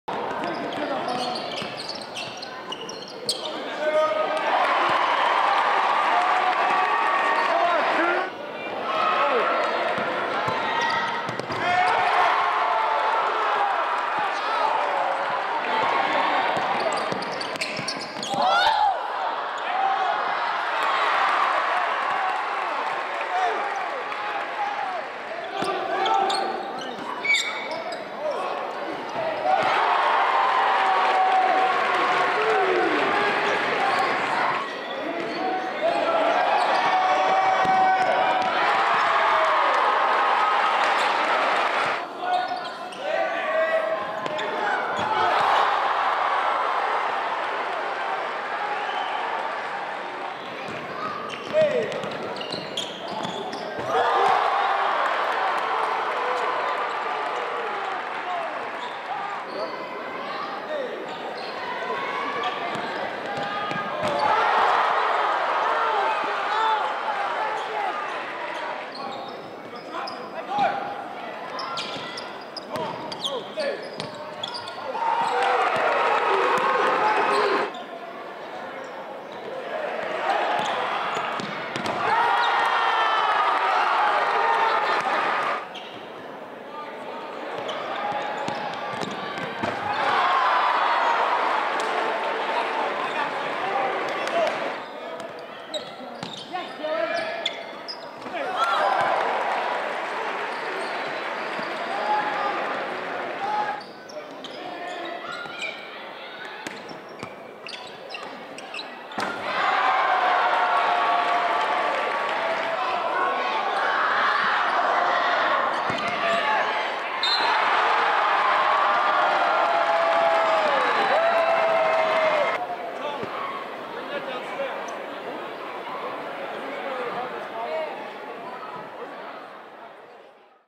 Brandon Randolph & Westtown KNOCKS DOWN Friends Central!! | RAW HIGHLIGHTS | Mars Reel
Arizona Commit Brandon Randolph lead the way for Westtown Prep (PA) with his 27 Point performance! 5-Star Center Mo Bamba scored 17 Points while 5-Star Jr. Forward Cam Reddish added 13 Points in the 82-59 WIN over Friends Central!

Join us as we explore the lives of the top NBA prospects through Mars Reel Chronicles. Additional series coming 2019!